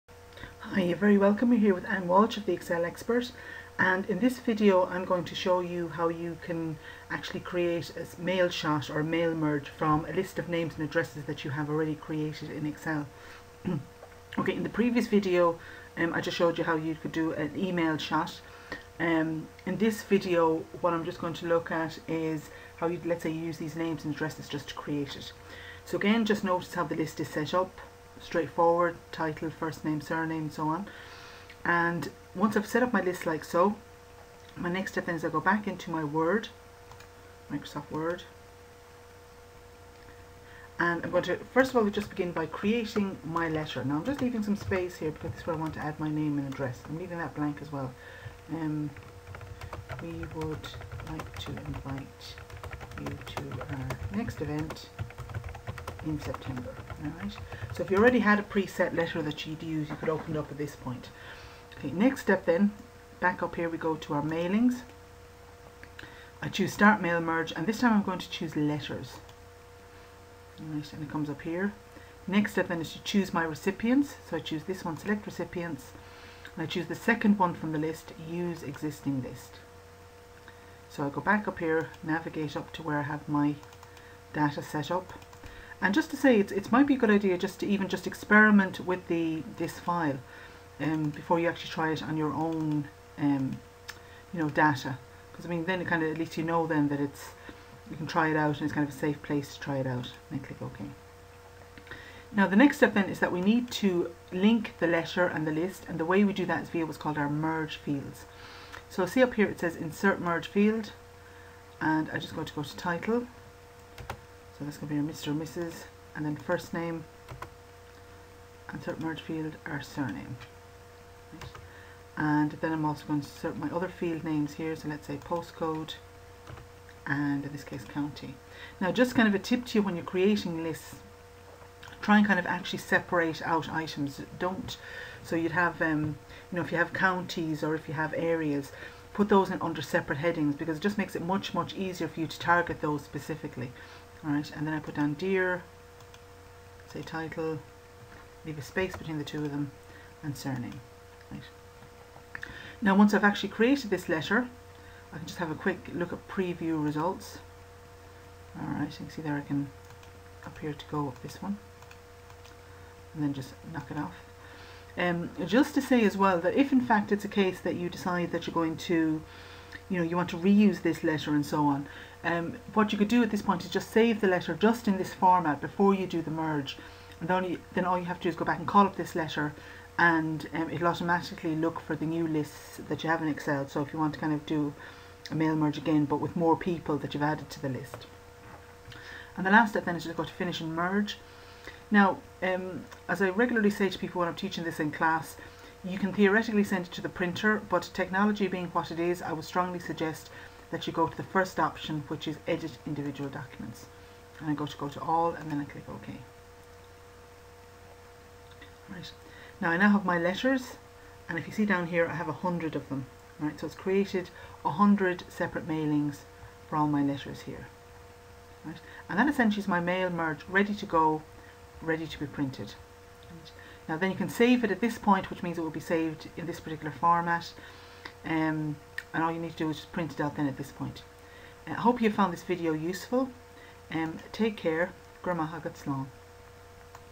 Creating a mail shot from an Excel list of names and addresses
Many people create their mailing lists in Excel. This video shows you how to use your Excel list with MS Word to quickly send letters and emails.